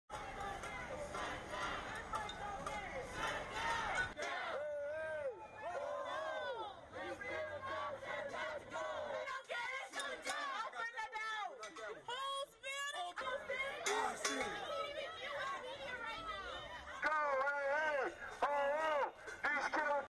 USA: "White lives matter, too!" Lone heckler crashes Mike Brown rally
M/S Protesters blocking a street intersection

M/S Protesters blocking a police car

C/U Protesters blocking The Plaza In Clayton Office Tower entrance

W/S Protesters blocking The Plaza In Clayton Office Tower entrance

M/S Police officers

W/S Protesters blocking a street intersection

M/S Protesters blocking a street

W/S Protesters marching

M/S Protesters marching

SOT, Deray McKesson, Ferguson and Twitter Activist (English): "I think not telling us when the announcement is going to come is a power play from the state, they could let us know. We are ready though, we've been ready, and we're protesting."

M/S Pro Darren Wilson protester

C/U Pro Darren Wilson Banner

SOT, Darren Wilson Supporter (English): "All lives matter, and black lives do matter, but so do white lives."

W/S Protesters marching

M/S Protesters marching

M/S Protesters marching

M/S Protesters marching

W/S Protesters marching

SCRIPT

Around 200 protesters gathered at Shaw Park in St Louis, Monday, to march for justice for Michael Brown, the unarmed African American youth slain by police in August.

Demonstrators gathered around Clayton, the governmental and financial district of the county of St. Louis, blocking traffic and obstructing the entrance to several buildings including The Plaza in Clayton Office Tower.

A small counter-protest in support of Mike Brown's killer Darren Wilson took place, with one woman telling press "Black lives do matter, but so do white lives."